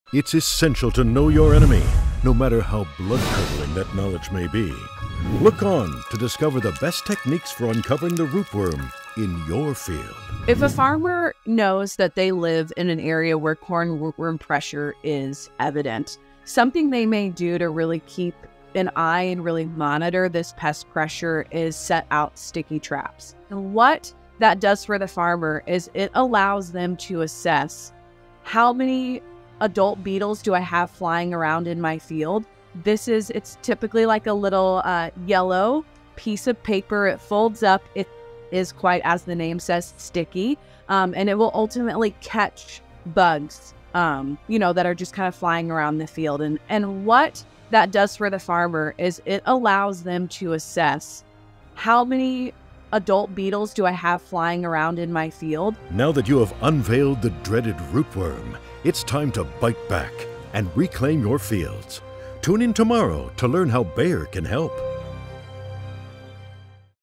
Rootworm Week: Episode 3- Best Techniques for Uncovering the Rootworm
Did you know that rootworms can cause up to $2 billion dollars of damage annually to the nation’s corn crop below ground?

This is why Bayer is focused on generating awareness of these nasty pests and partnering with farmers and arming them with the tools, technology and best practices to fight rootworm.